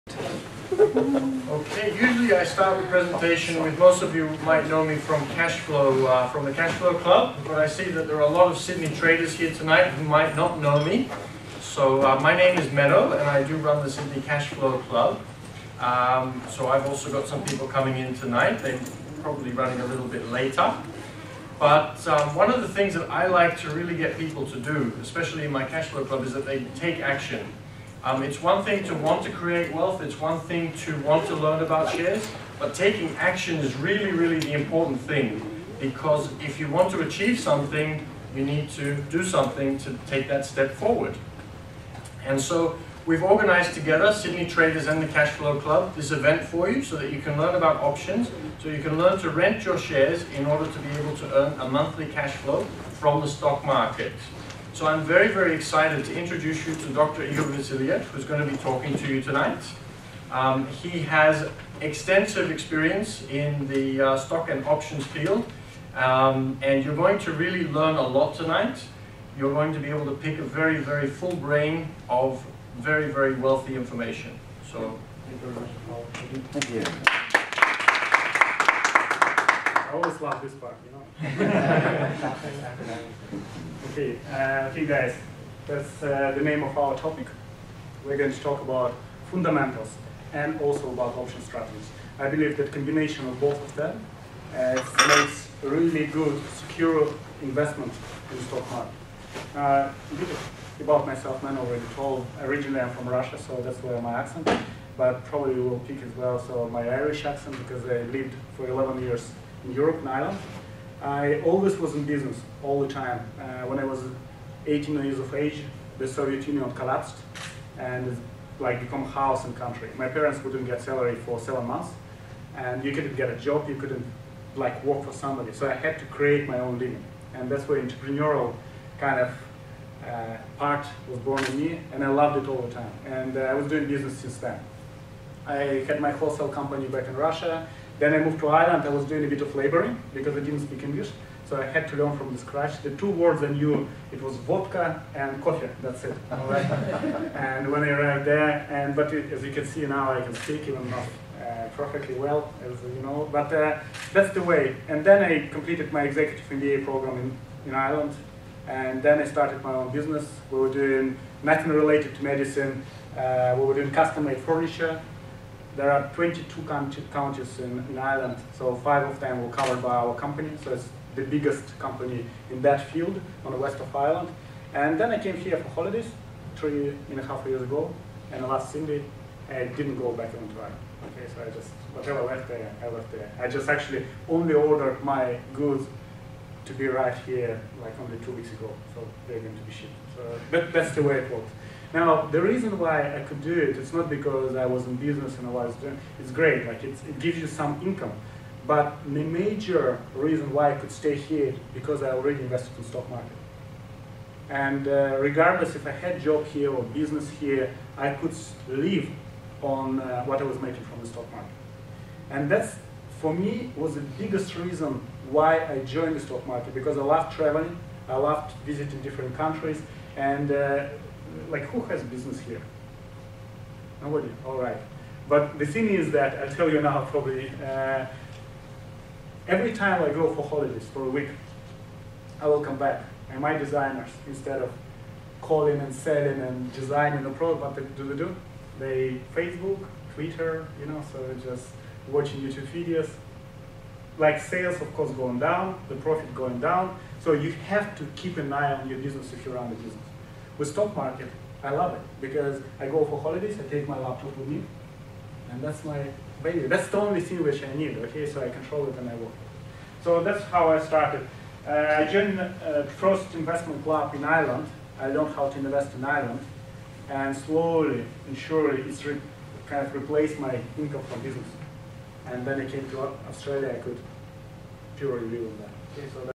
Double your income part 1
During this video:
Igor Vasiliev - introduction
Risks and rewards on the Stock Market,
Internal and External reasons for rapid price changes
Important traits of successful investors